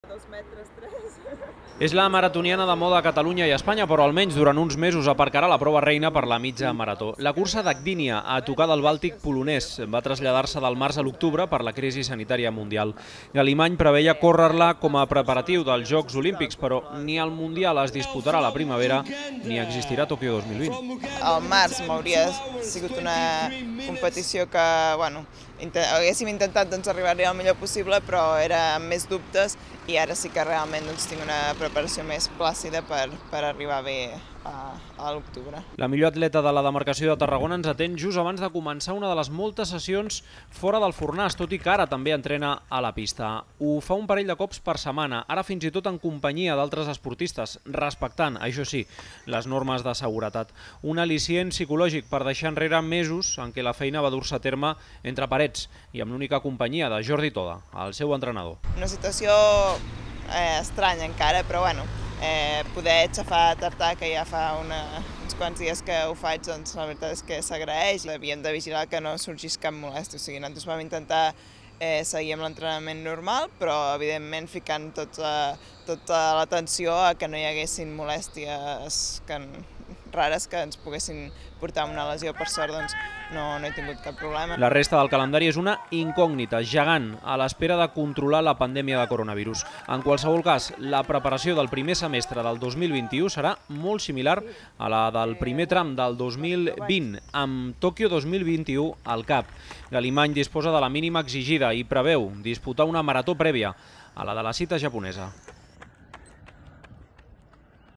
Pròxima parada: Mundial de Mitja Marató
Marta Galimany disputarà la cita que se celebrarà a Polònia el mes d'octubre. Serà la primera gran trobada atlètica internacional després de l'aturada competitiva i de l'ajornament dels Jocs Olímpics del 2020 on la vallenca té plaça assegurada per a la marató.